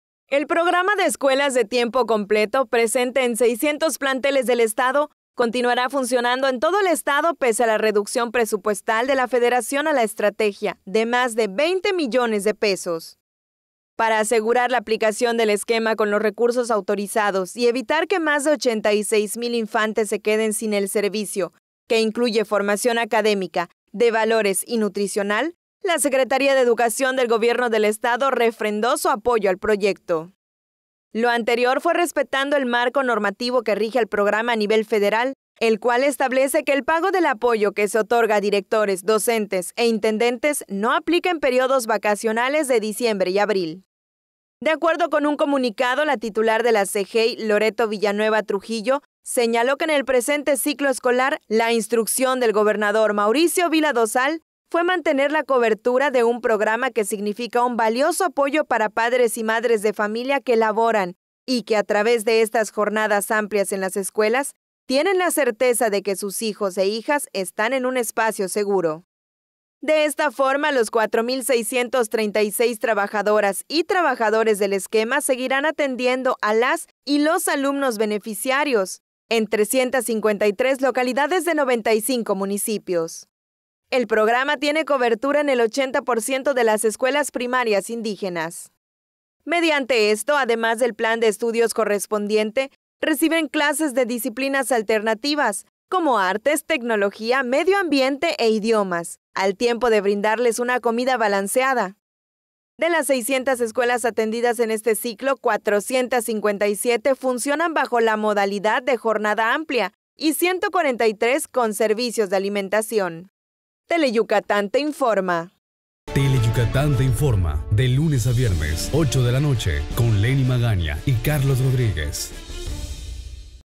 Educación I Programa Escuelas de Tiempo Completo sigue operando. | Tele Yucatán
Educación I Pese al recorte de presupuesto de 20 mdp, el Programa de Escuelas de Tiempo Completo está presente y sigue operando en 600 planteles en el estado.

Las noticias más recientes de Yucatán.